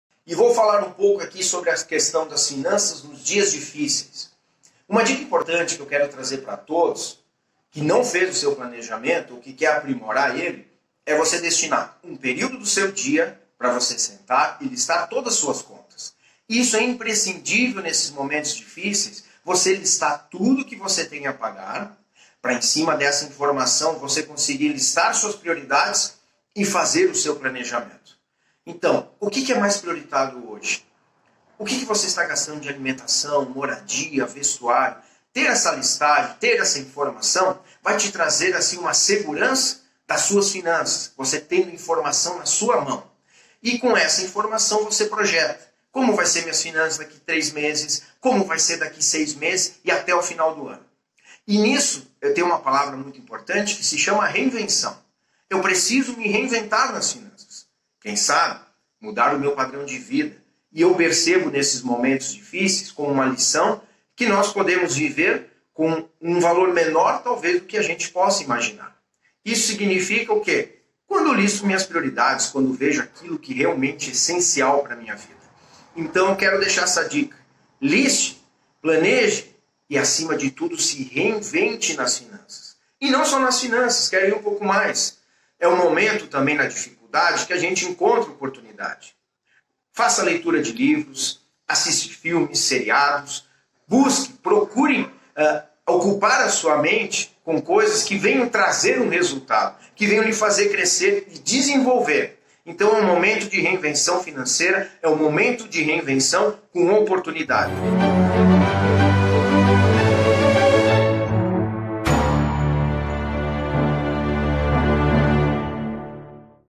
Coach Financeiro Natanael Buogo fala sobre economia em tempos de crise
Nesse momento de crise na saúde pública e na questão econômica é preciso alguns cuidados com nossas finanças. Acompanhe algumas dicas do Coach Financeiro Natanael Buogo